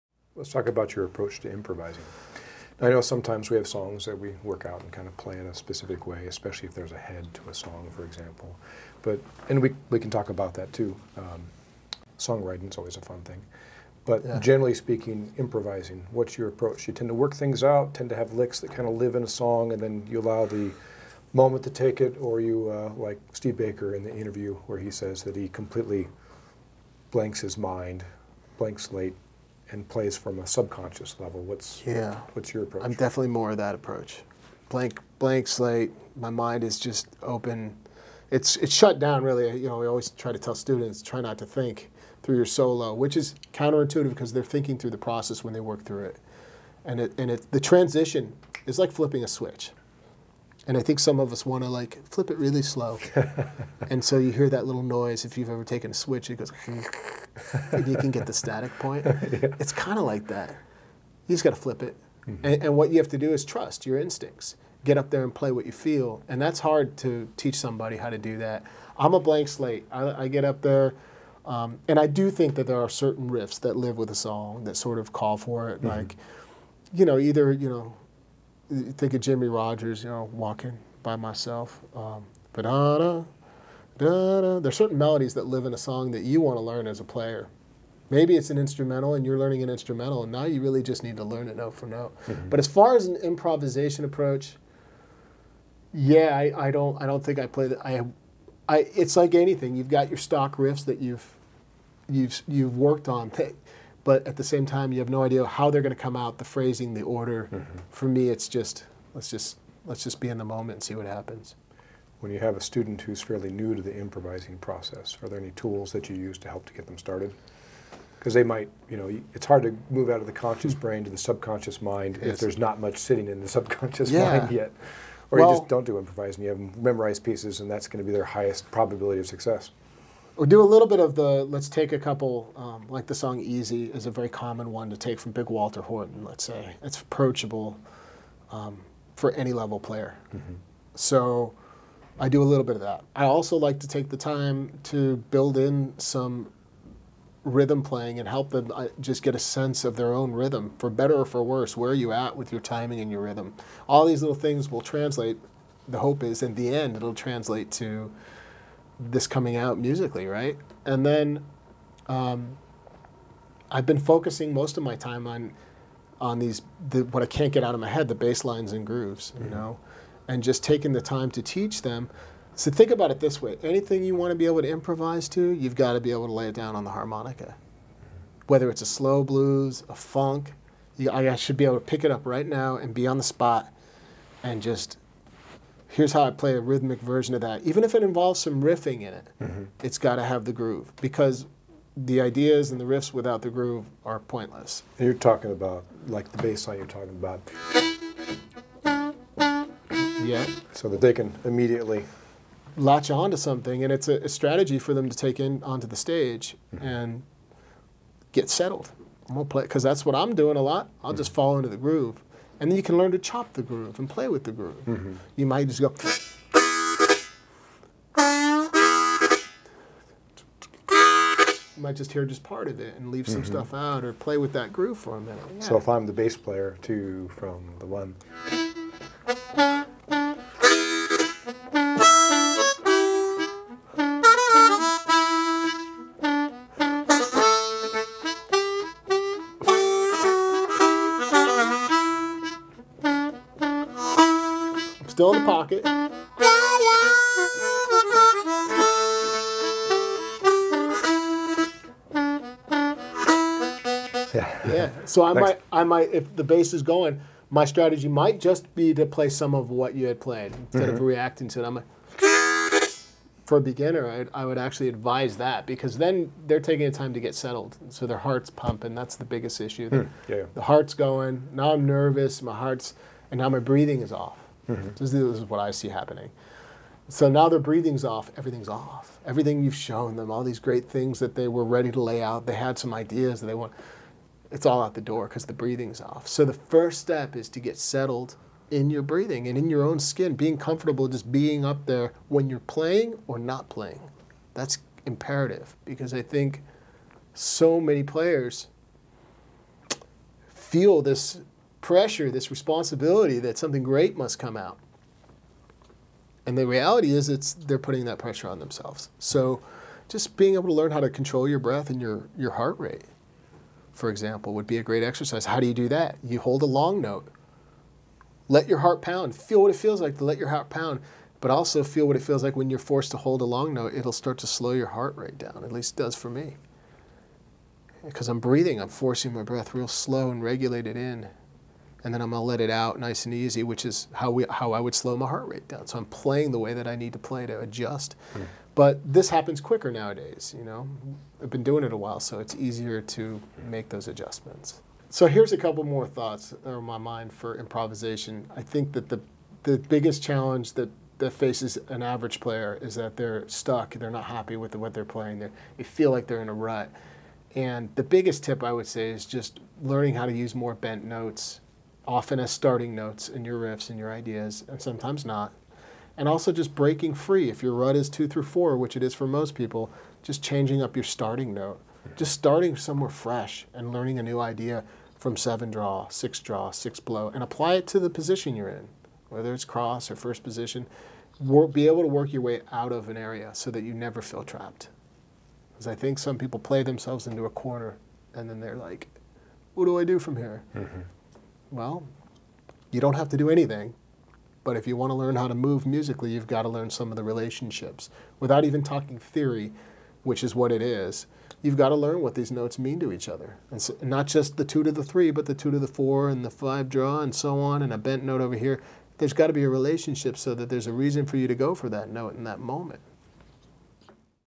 Ronnie Shellist's Approach to Improvising with David Barrett
Join me for a segment of my interview with blues harmonica player Ronnie Shellist. In this video Ronnie shares his approach to improvising.